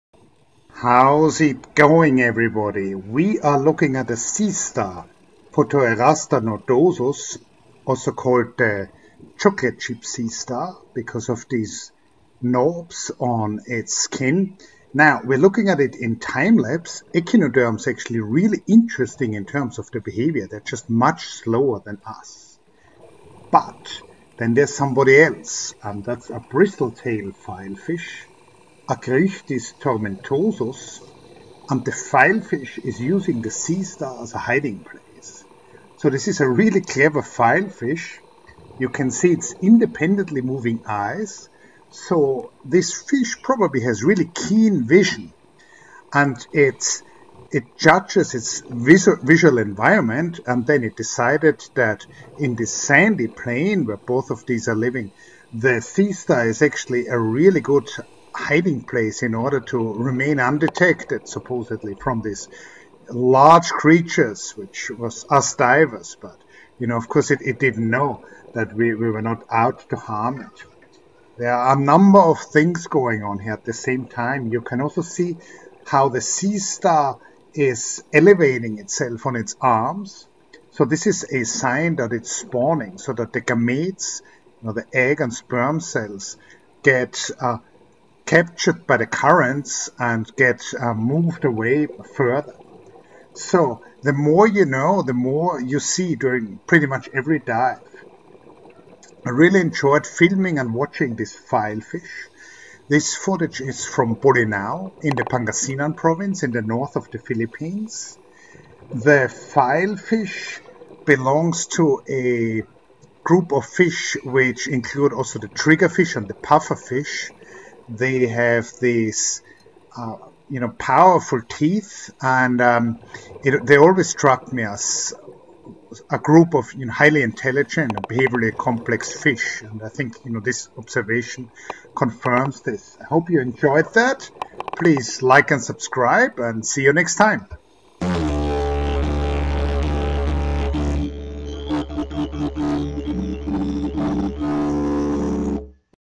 Filefish Hiding Around Sea Star
Protoreaster nodosus, chocolate chip sea star serving as a hiding spot for a bristle tail filefish, Acreichthys tomentosus. Bolinao, Pangasinan, Philippines, filmed on scuba in their natural environment with an Olympus EM1.